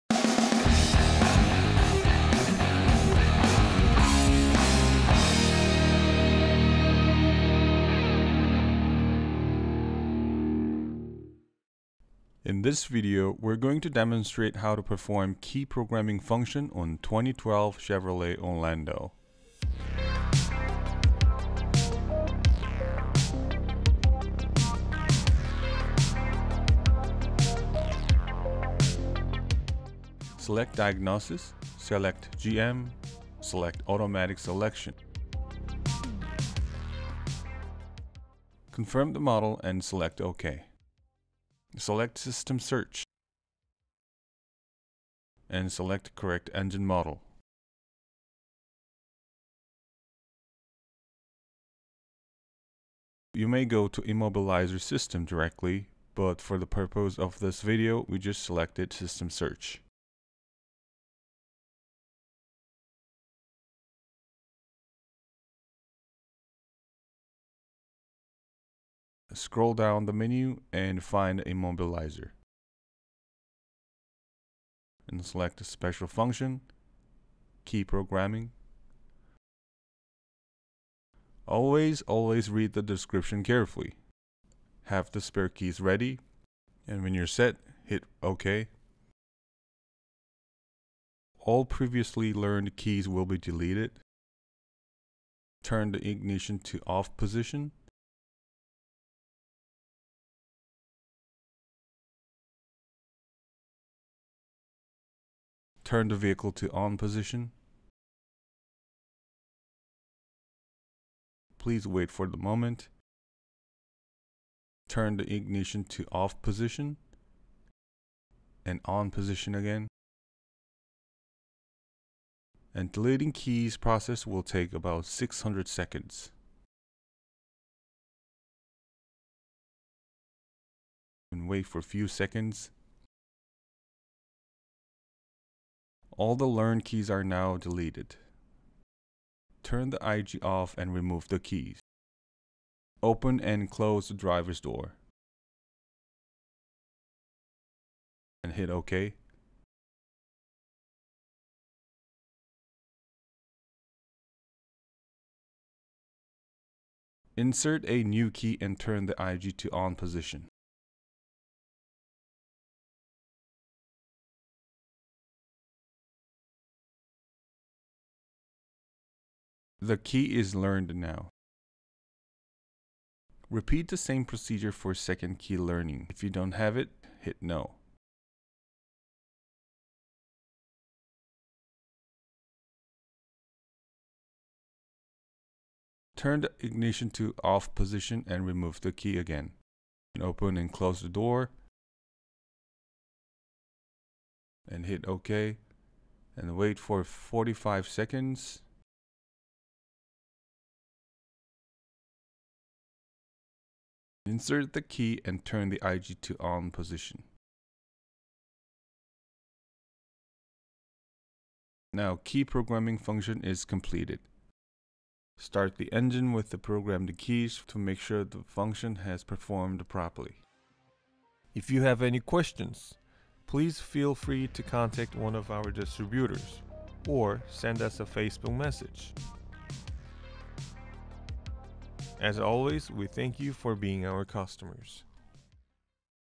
Key Programming on 2012 Chevrolet Orlando
This video demonstrates how to perform 'Key Programming' function on 2012 Chevrolet Orlando's Immobilizer system. Hope it helps!